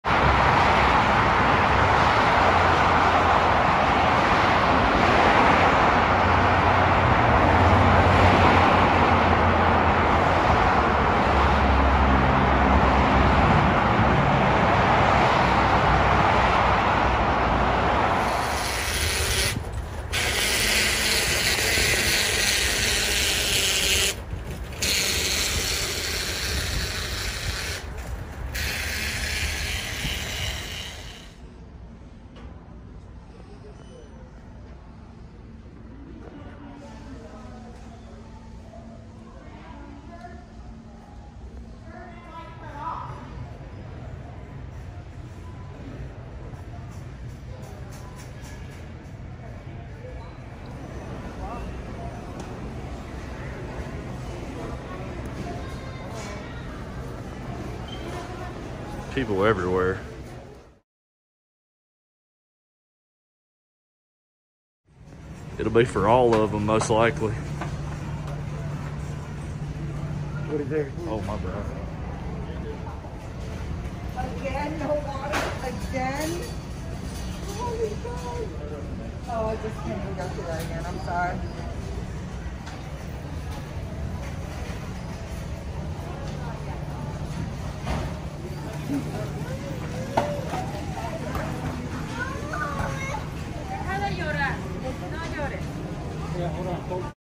3-11-2021 Denver, CO Historical Winter Storm Imminent - Chaos in Stores - Empty Isles
Chaos has ensued across portions of the Denver metro area as a major winter storm is forecast to impact the region going into the weekend.  Numerous residents packed out the isles ad the local grocery stores to get their hands on last minute supplies before the snow sets in.  Supplies of frozen goods, water, and many dry goods were already running pretty bare as people are expecting business closures for at least a day or two due to the storm.